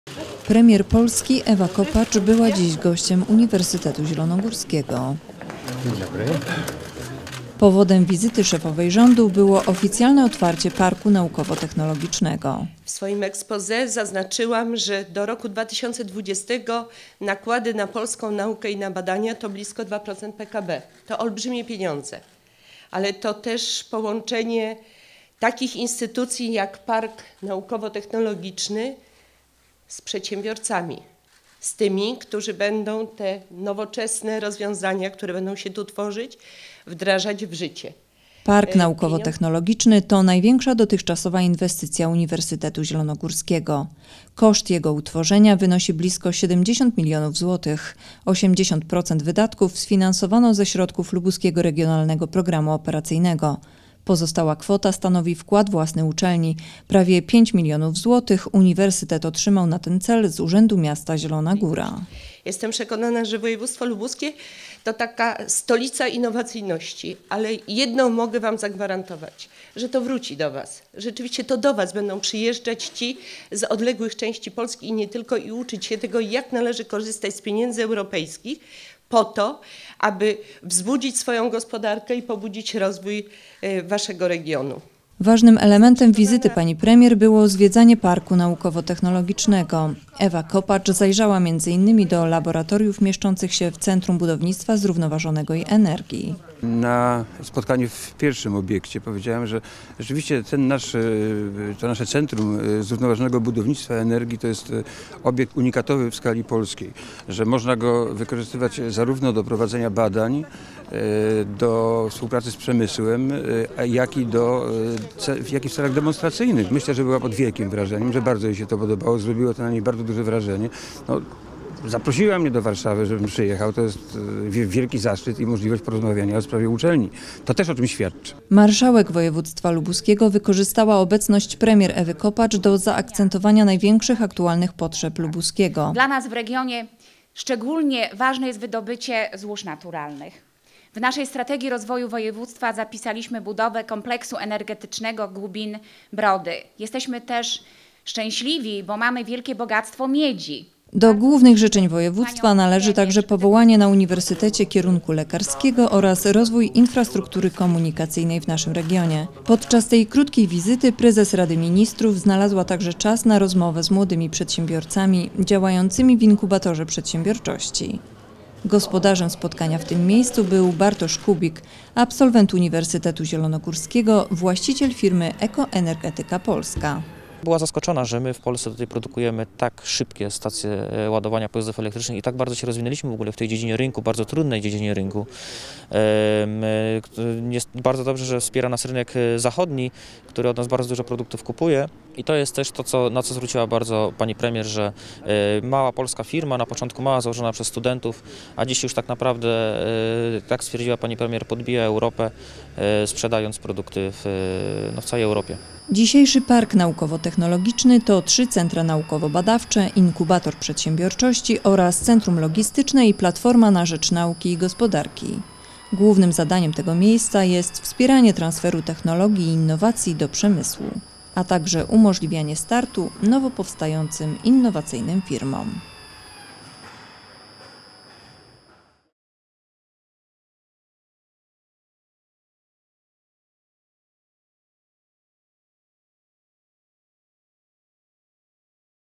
Premier Ewa Kopacz otwiera Park Naukowo-Technologiczny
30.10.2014
Premier RP Ewa Kopacz była dziś gościem Uniwersytetu Zielonogórskiego. Powodem wizyty szefowej rządu było oficjalne otwarcie Parku Naukowo-Technologicznego UZ.
Park to największa dotychczasowa inwestycja UZ. Koszt jego utworzenia wynosi bliko 70 mln. zł. 85 procent wydatków sfinansowano ze środków LRPO. Pozostała kwota stanowi wkład własny uczelni. Prawie 5 mln zł Uniwersytet otrzymał na ten cel z Urzędu Miasta Zielone Góra.
Producent:
Telewizja UZ